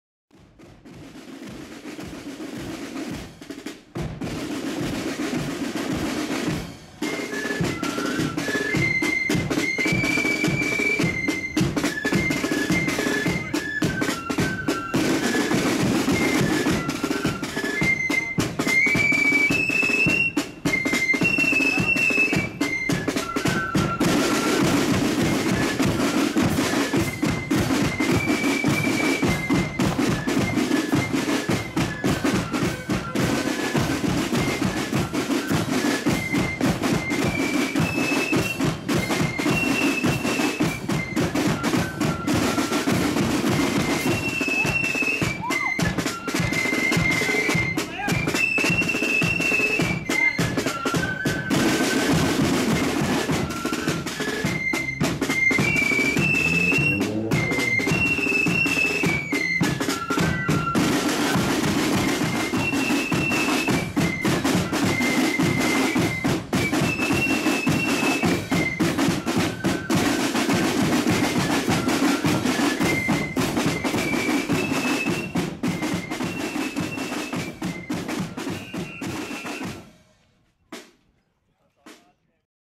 Carrick Defenders (2)1st August 2020
Carrick Defenders on the march in Eden Carrickfergus Co Antrim